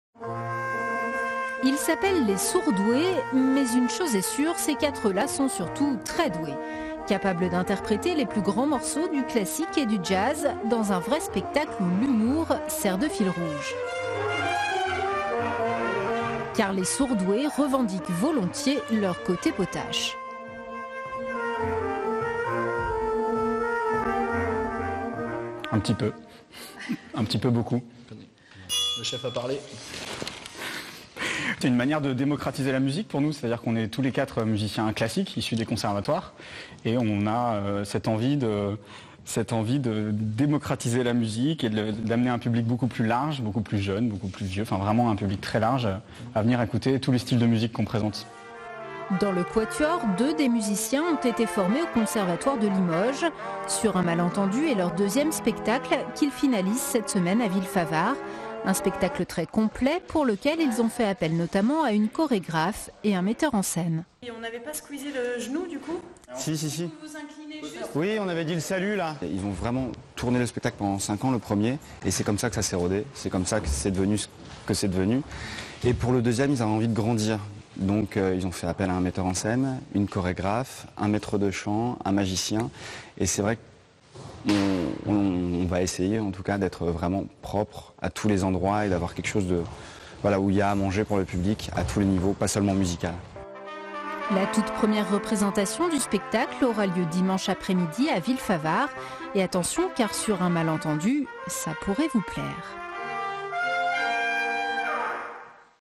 Les Sourds-Doués - France 3 Limousin
Reportage du 03/09/2016
Création de "Sur un malentendu..." à la Ferme de Villefavard en Limousin.